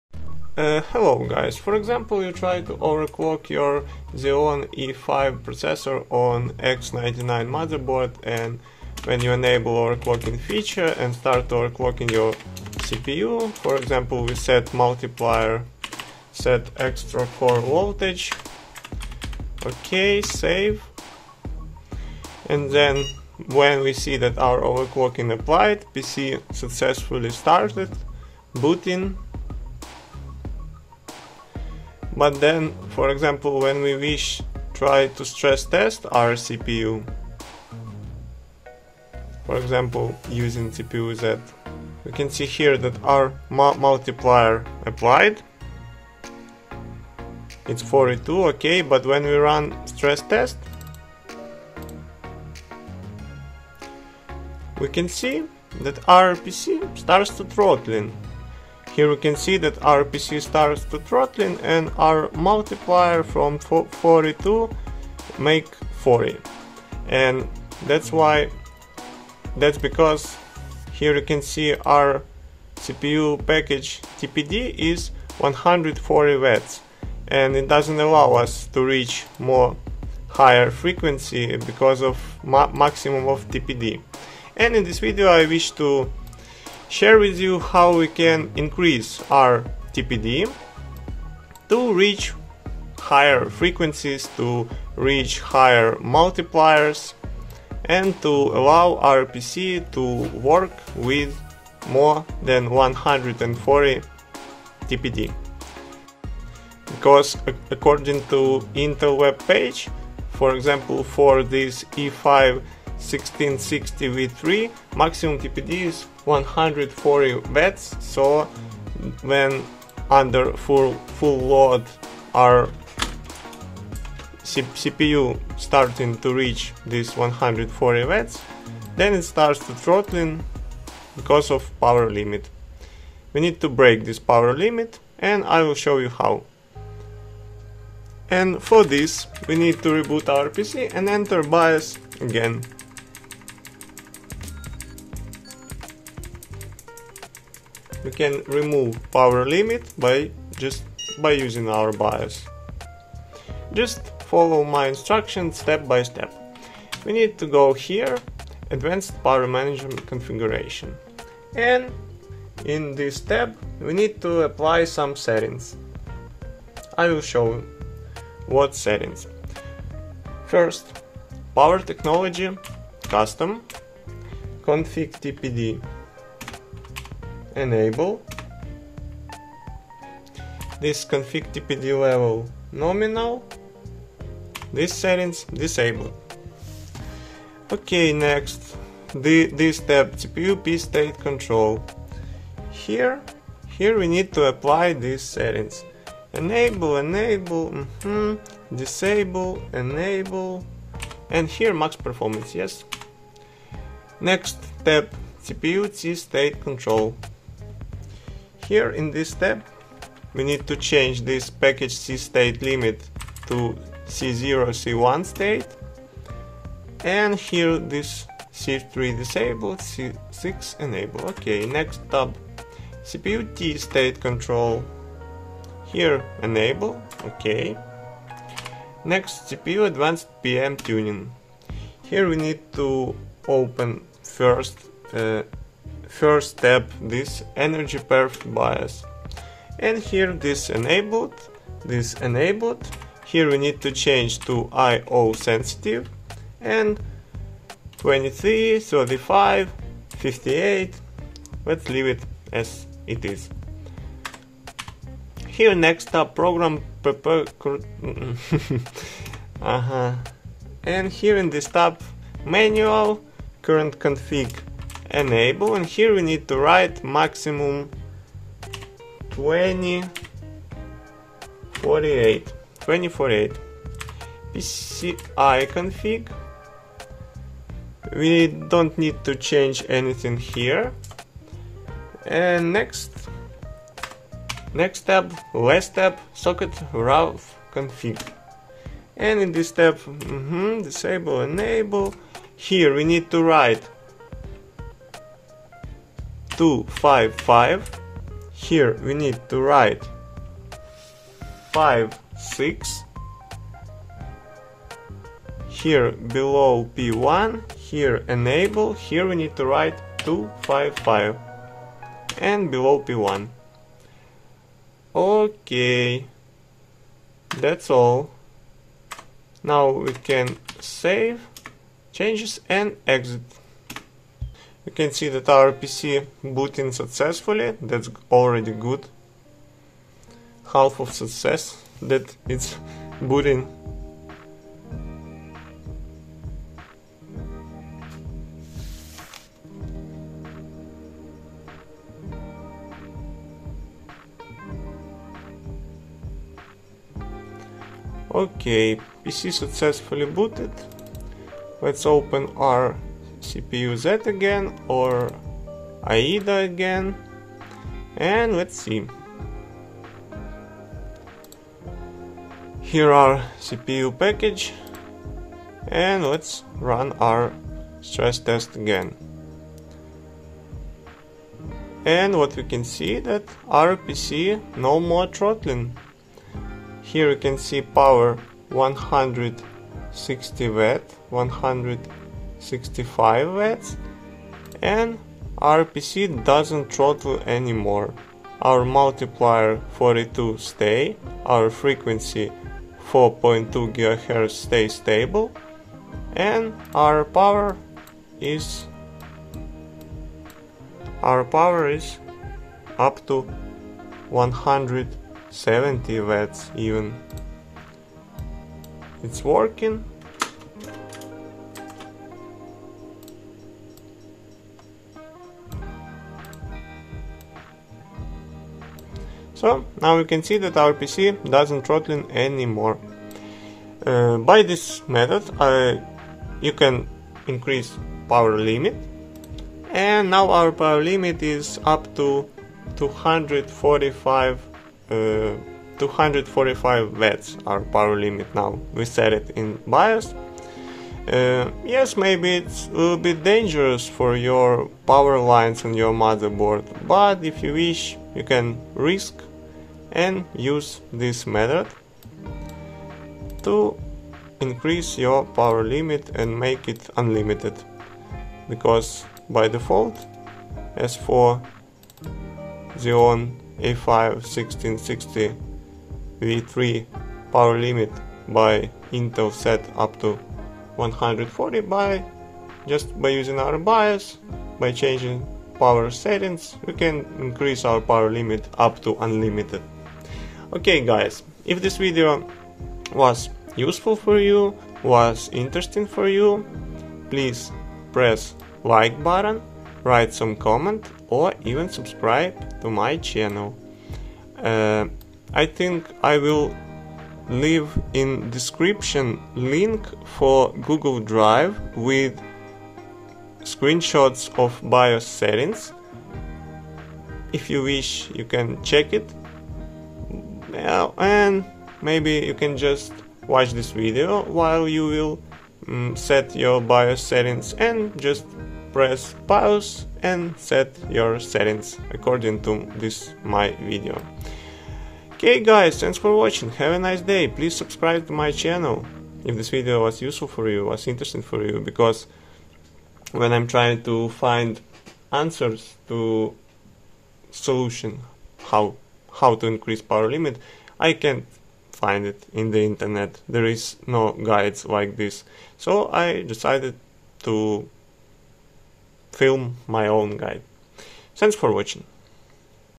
x99 Huananzhi INCREASE CPU POWER LIMIT - UNLIMITED TDP | OVERCLOCKING Xeon E5 1660 V3 / 1650 / 1680
In this video I want to show you HOW TO INCREASE CPU POWER LIMIT and make it UNLIMITED TDP on X99 motherboard, such as HUANANZHI, JINGSHA, MACHINIST e.t.c.. for OVERCLOKING such CPU as XEON E5 1660-V3, 1650-V3, 1680-V3 on LGA 2011-3 socket! How to OVERCLOCK CPU at higher frequencies! CORRECT BIOS SETTINGS for XEON OVERLOCKING!